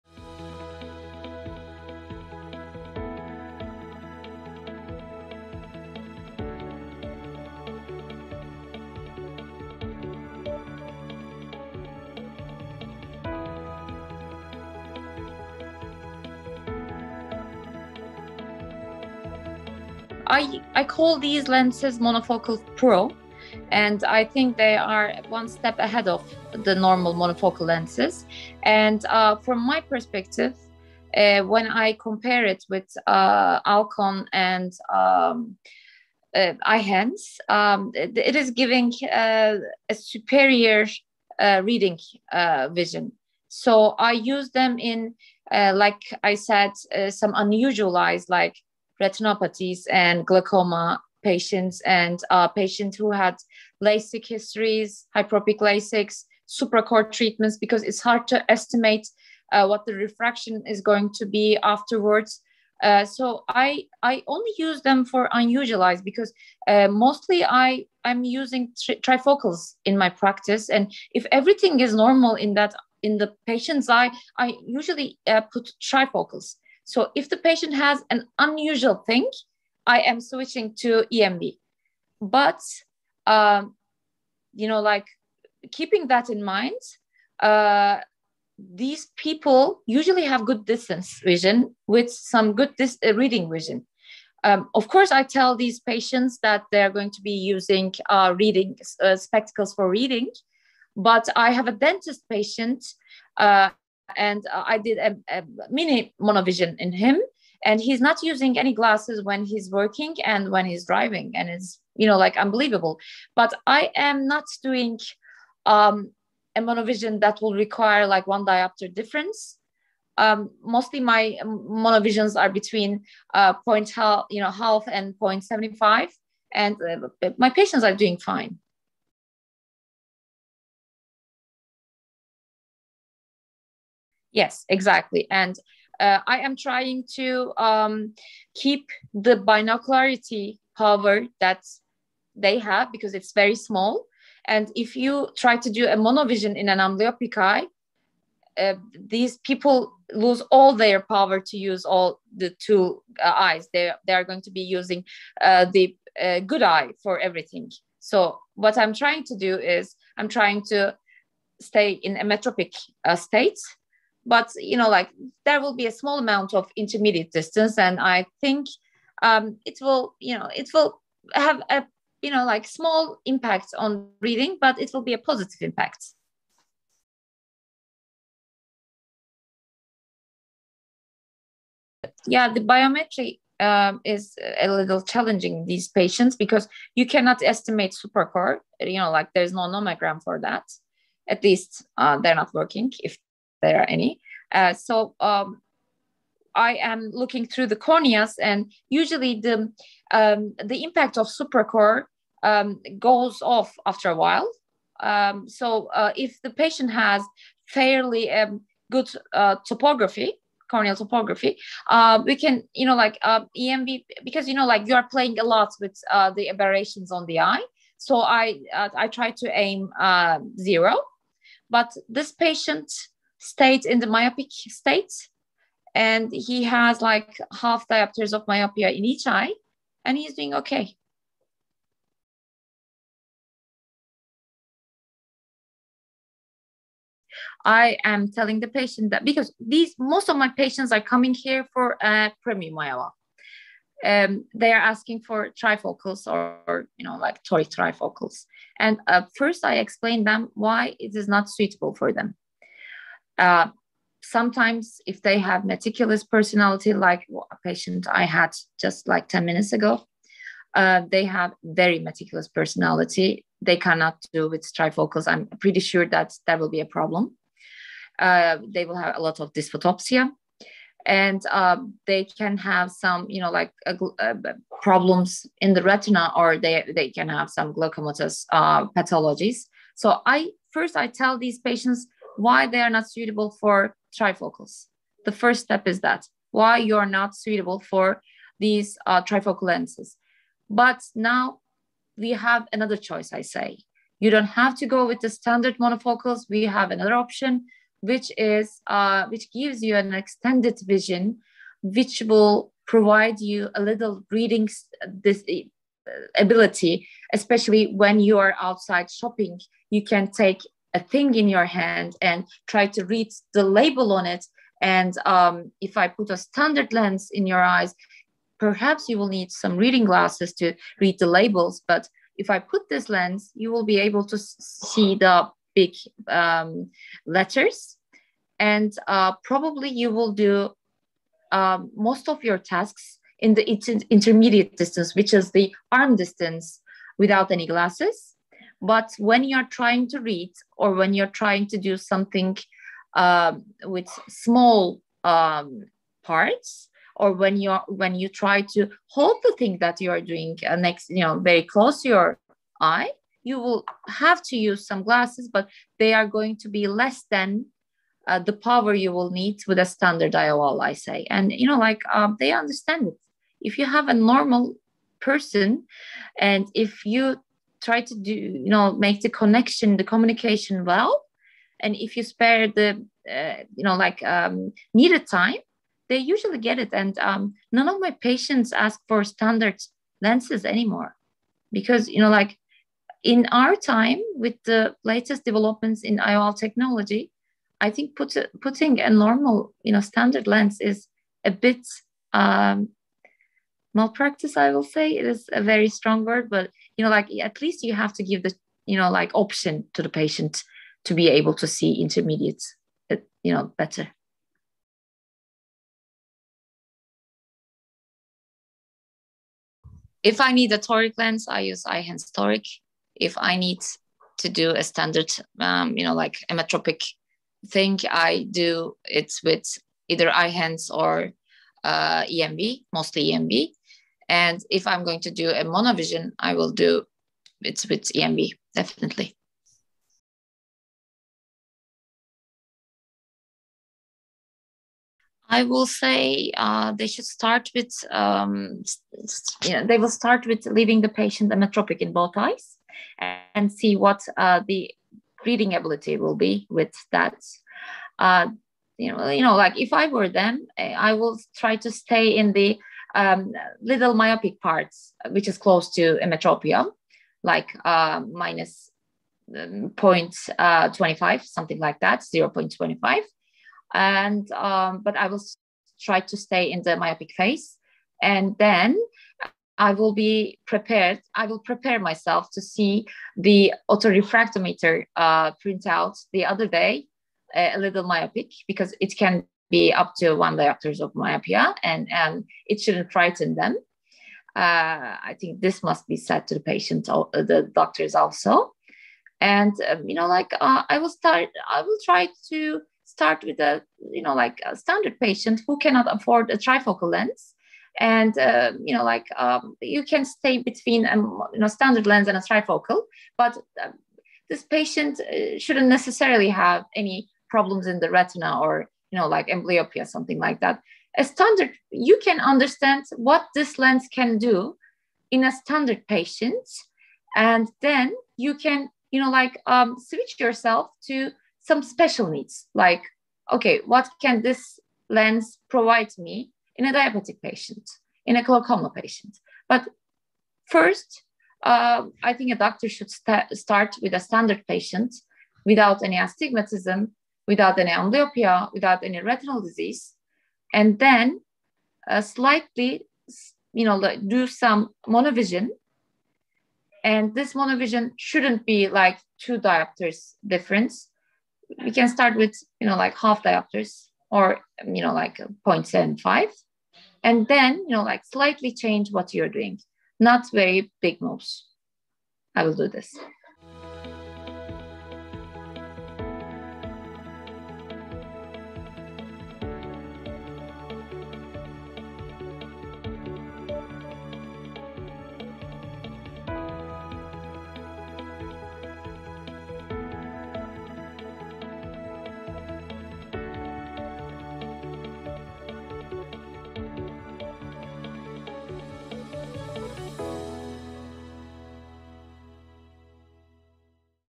Plus1 EMV | RayOne EMV patient suitability with Dr Basak Bostanci
Leading Turkish ophthalmic surgeon, Dr Basak Bostanci, shares her thoughts on which patients are most suitable for RayOne EMV in her premium practice.

Not all Rayner products are approved for sale in every country.

0:00 Introduction
0:19 Why is RayOne EMV different to other monofocal lenses?
02:11 With an amblyopic eye, are you aiming for good distance vision?
03:54 How do you describe RayOne EMV to your patients?
06:45 What lenses do you use in your practice?
07:14 What advice would you give to surgeons thinking of using RayOne EMV?